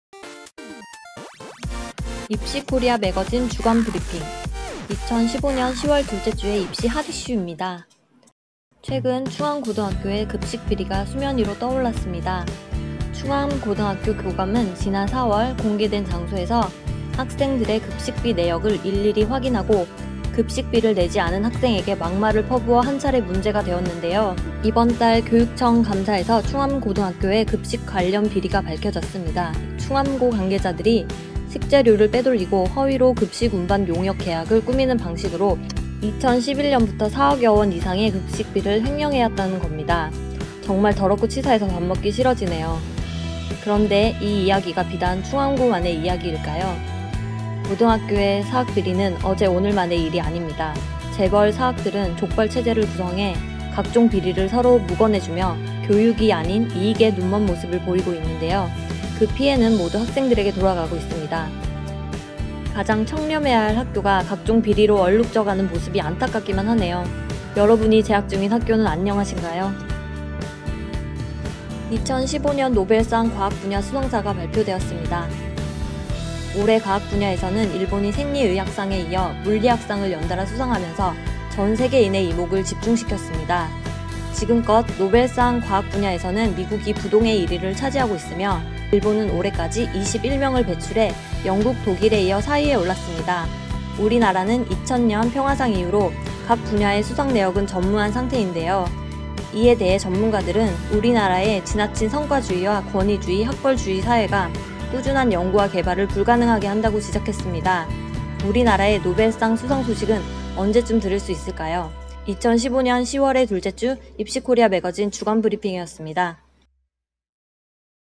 [입시코리아매거진] 2015년 10월 둘째 주 주간브리핑(Weekly briefing)
한 주간의 핫 이슈를 정리해드립니다.

[2015년 10월 둘째 주]
* 충암고등학교 급식비리
* 2015 노벨상 과학분야 수상자 발표